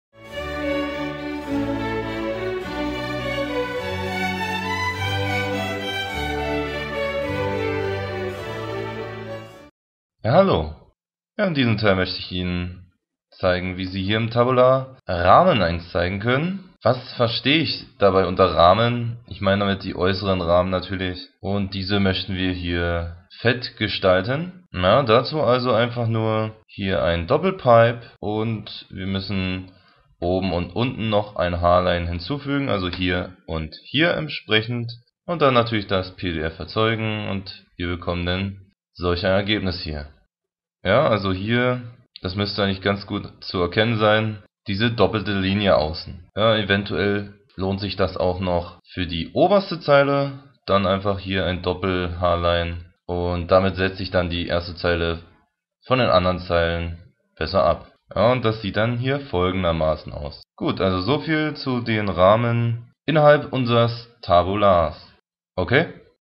Tabular: Rahmen - (LaTeX-1-058)
In diesem Tutorial wird Ihnen ein weiteres Video der LaTeX-Serie gezeigt. Wenn doppelte Außenlinien gewünscht sind, ist dies natürlich auch ganz einfach möglich.